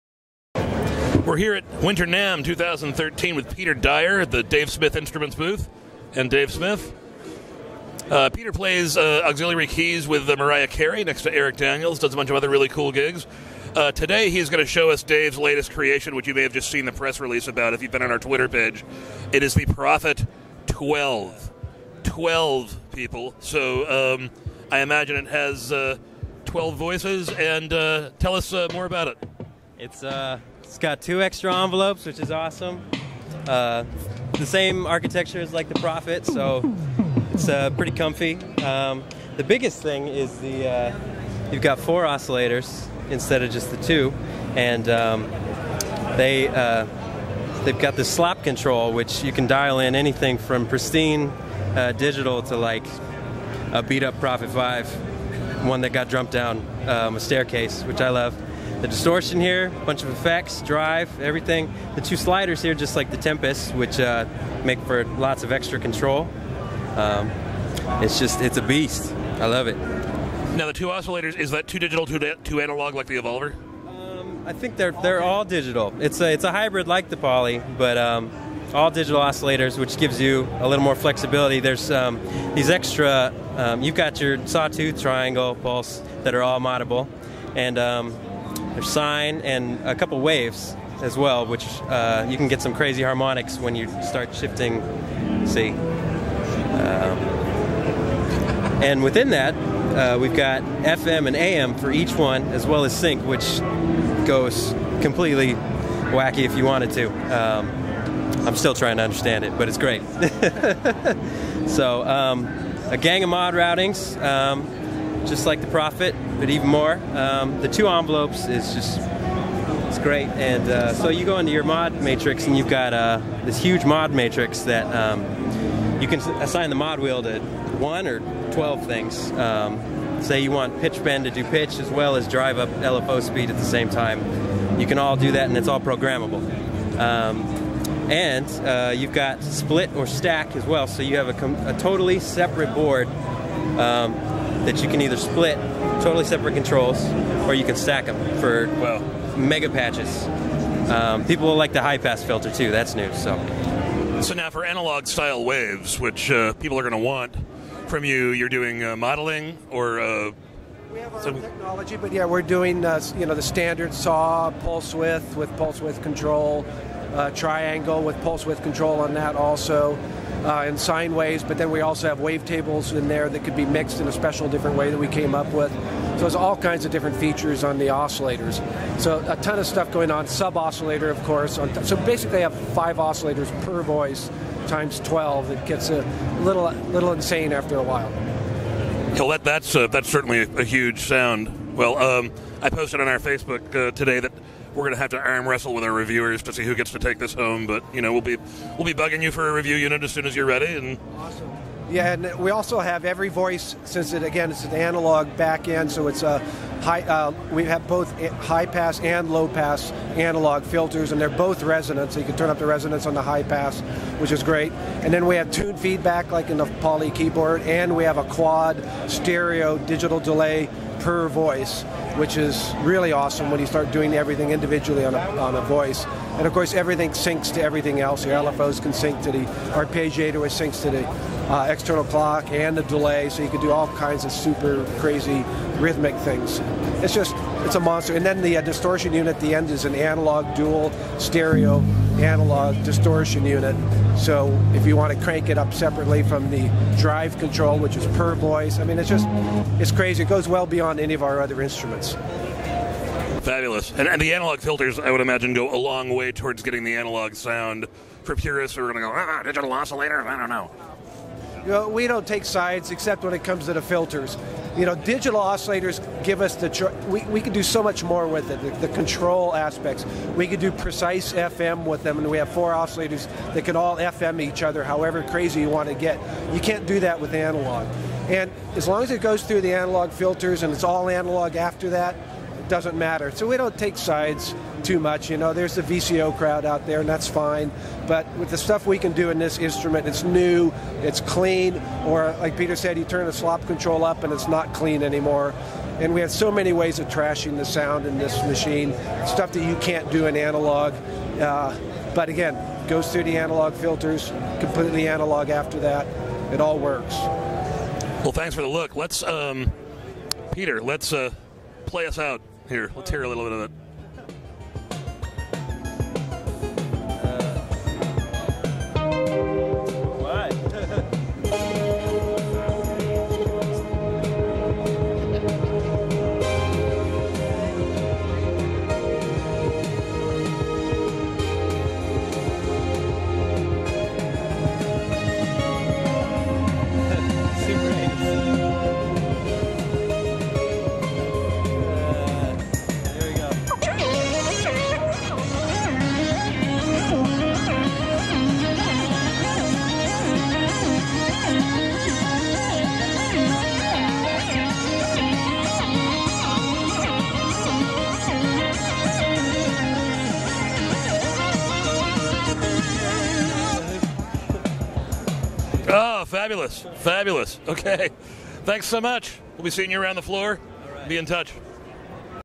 Keyboard magazine hands-on with the new Prophet 12 by Dave Smith at NAMM 2013
The Prophet-12 is a hybrid synth: four digital oscillators (that are capable of analog modeling as well as wavetable and vector-appropriate digital waveforms), but analog filters and envelope section. In Dave Smith's own words, it's the best synth he's ever designed. Find out why he thinks so as editor Stephen Fortner goes hands on.